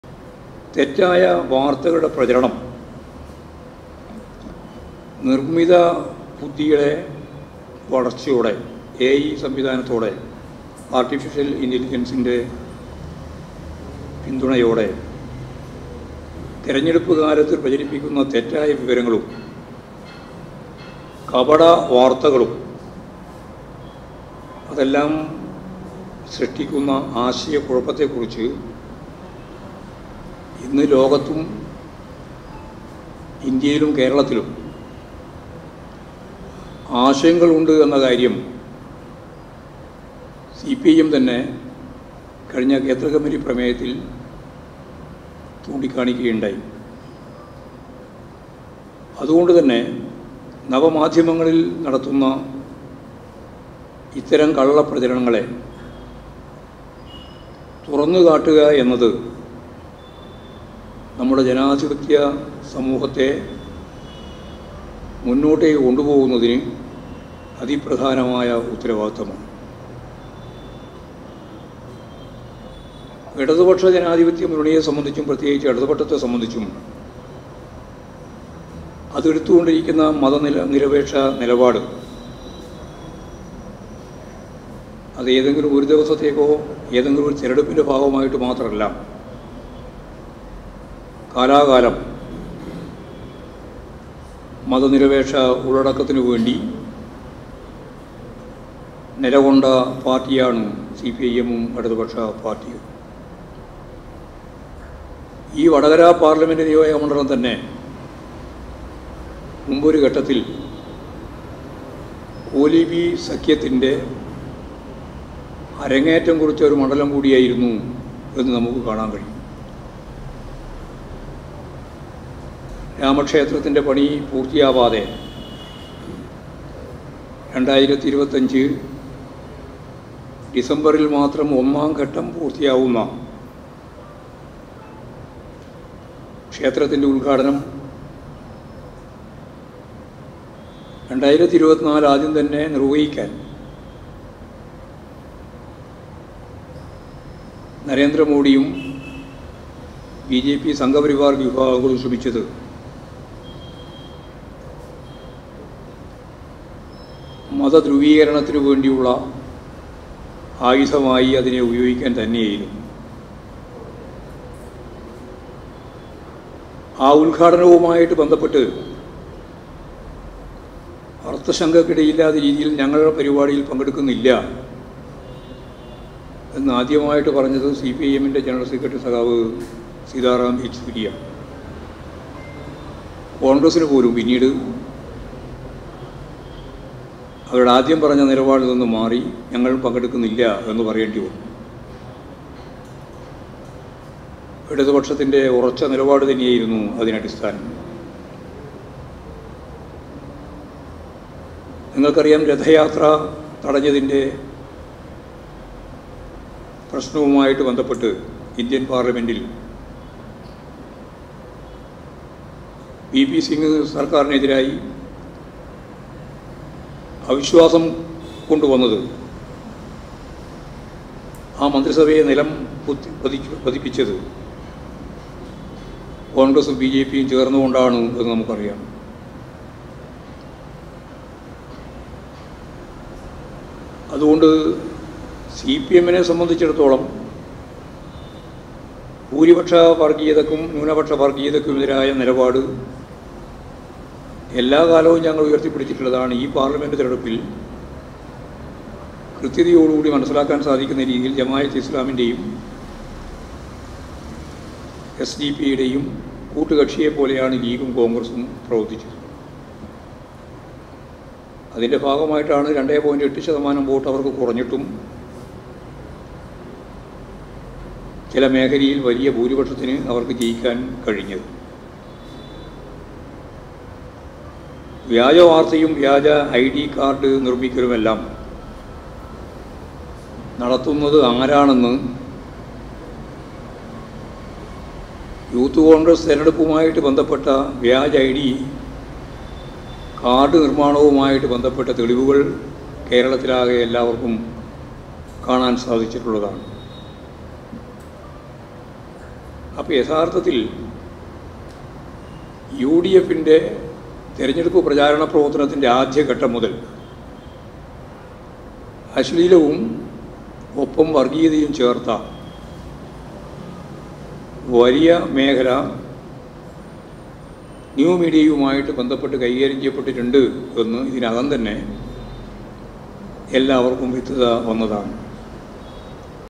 'വ്യാജ പ്രചാരണങ്ങളെ തുറന്നുകാട്ടി CPIM മുന്നോട്ടുപോകും'; എം വി ഗോവിന്ദൻ മാസ്റ്റർ | Govindan Master
'വ്യാജ പ്രചാരണങ്ങളെ തുറന്നുകാട്ടി CPIM മുന്നോട്ടുപോകും; തെറ്റായ വാർത്തയുടെ പ്രചാരണം AI സംവിധാനത്തോടെ കൂടുതൽ സജീവമായി'; എം വി ഗോവിന്ദൻ മാസ്റ്റർ
'CPIM will go ahead by exposing fake campaigns'; MV Govindan Master